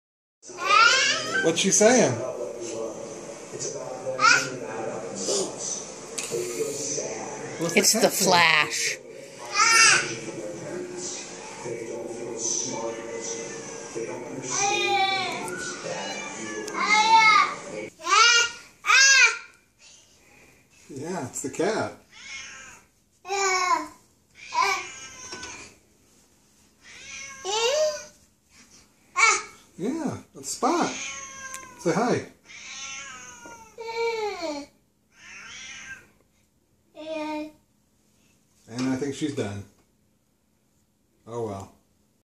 Daniel and Spot talking
Daniel talking back to the cat